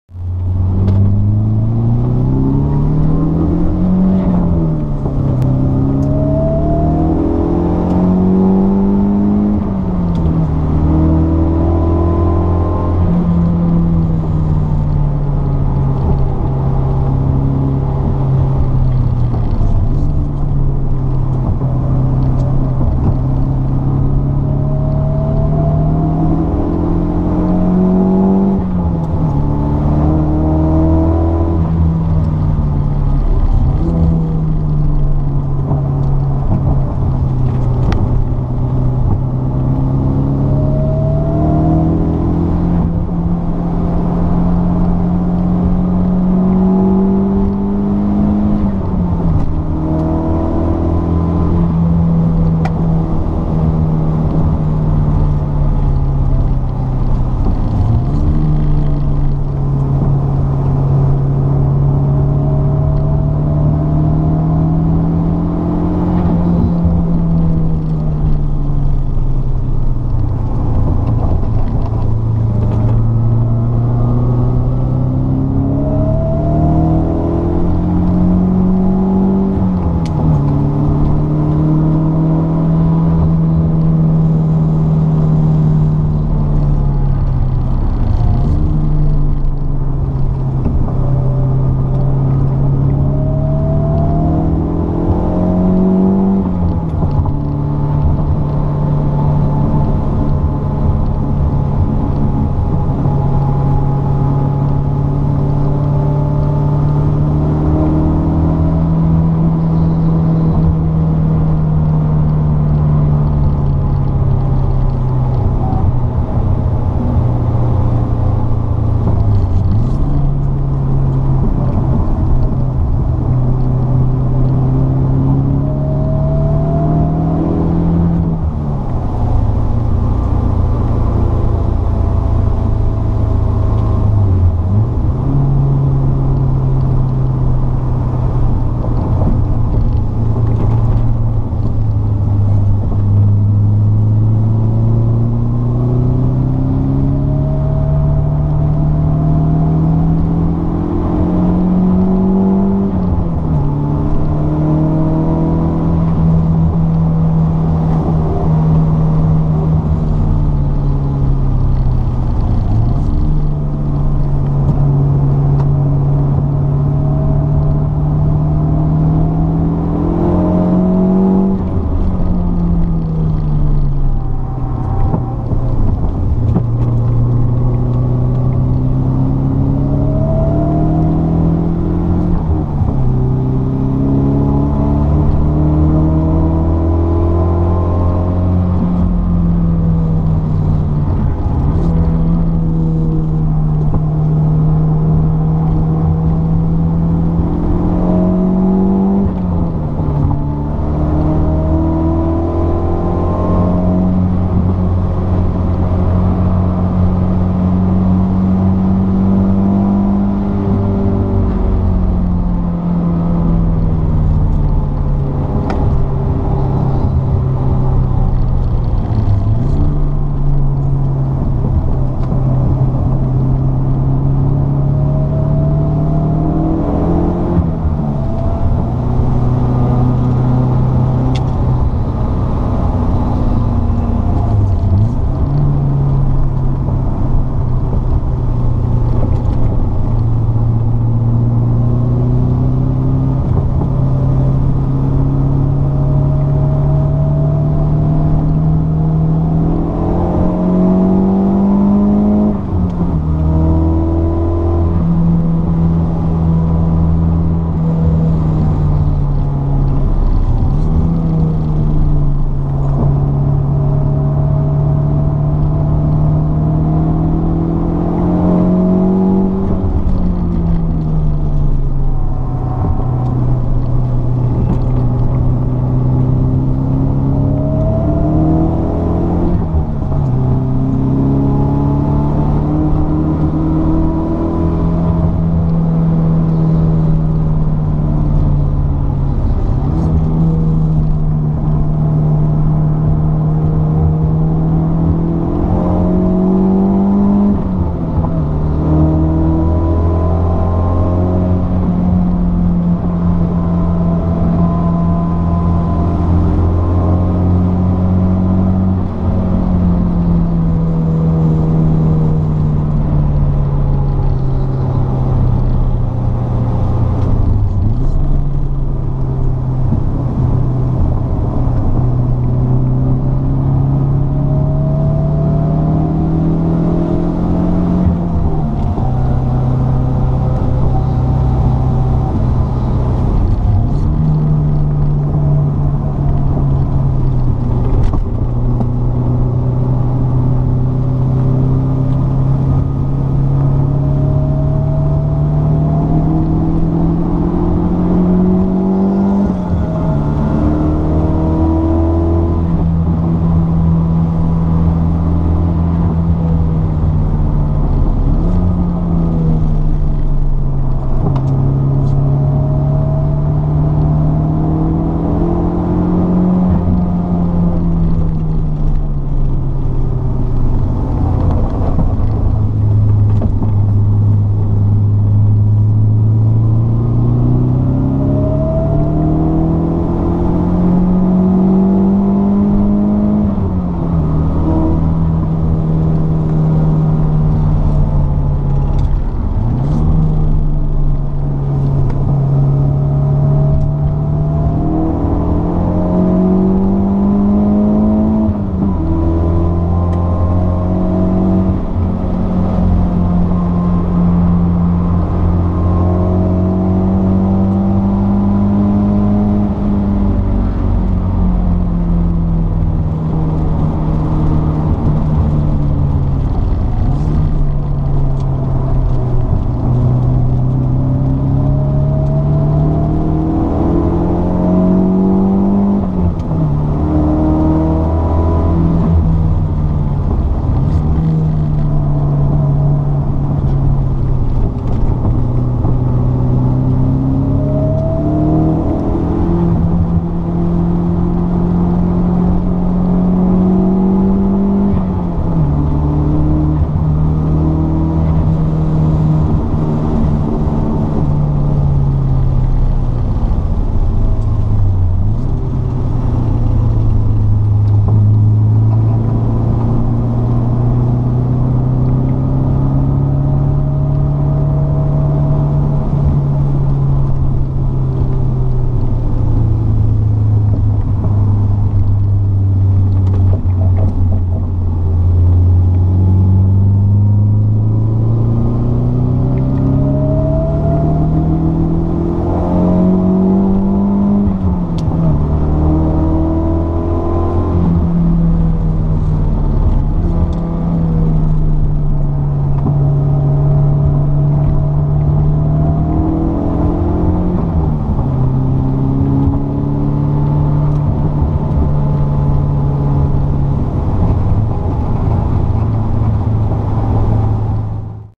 Triumph Acclaim TypeR
Following Dan's Triumph Acclaim TypeR at the TR Register trackday at Castle Combe